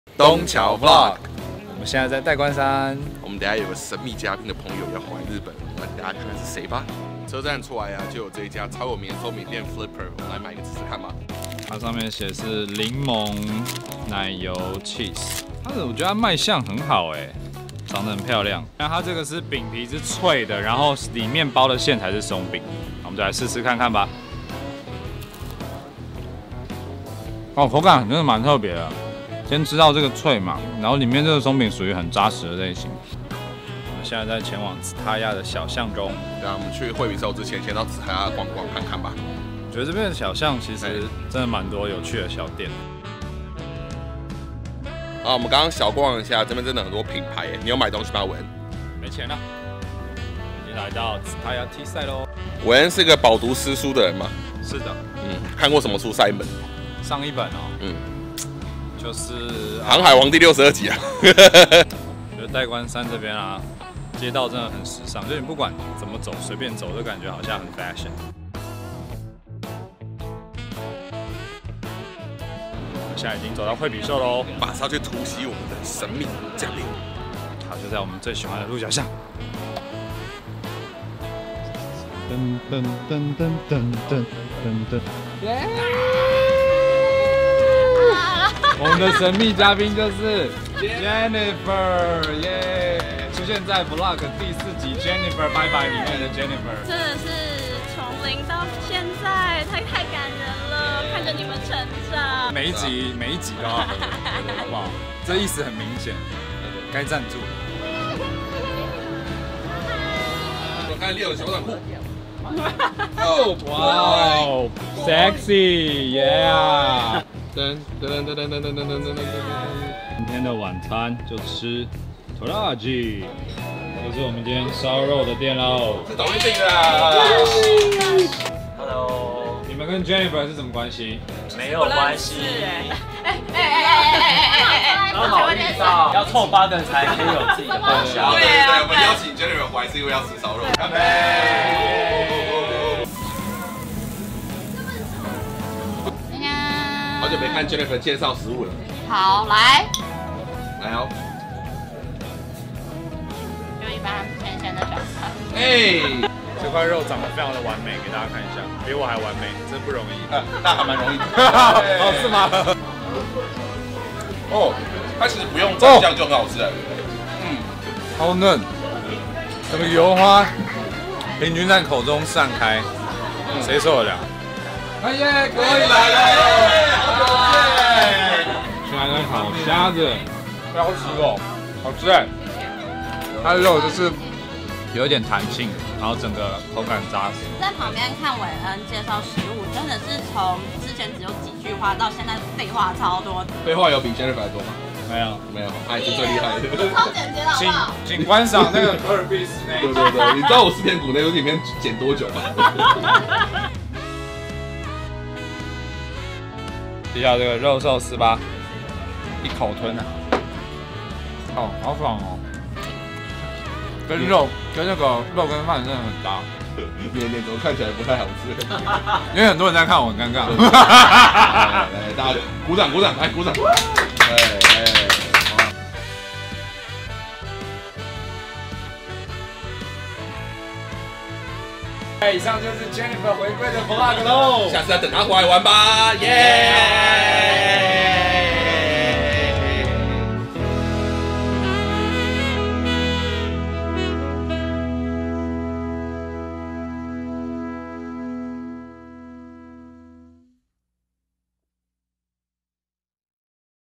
[東瞧VLOG]我們在惠比壽跟神秘嘉賓吃Toraji燒肉!
我們最愛的好朋友回來了！如果你Follow東瞧一陣子了，那你應該就知道是誰😏
讓我們繼續看下去，btw那個燒肉超好吃